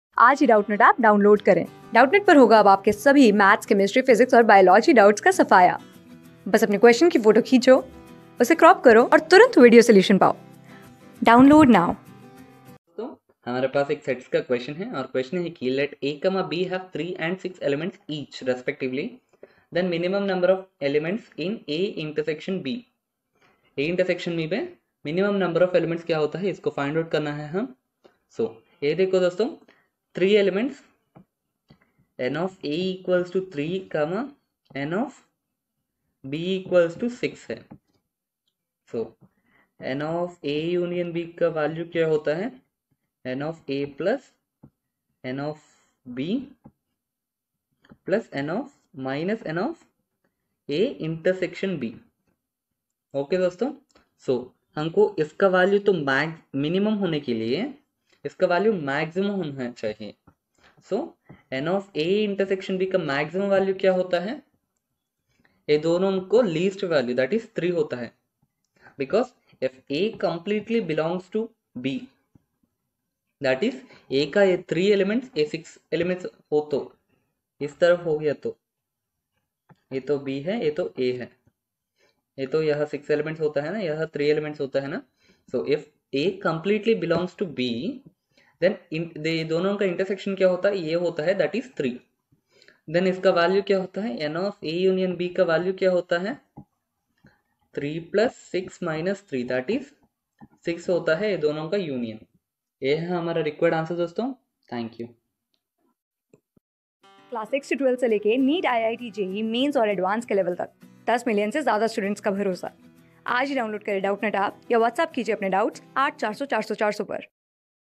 LetA andB have3 and 6 elements respectively, whatcanbe theminimum numberofelements in A cup B?
Class: 11
Subject: MATHS
Chapter: SETS
Board:IIT JEE

👉 Have Any Query? Ask Us.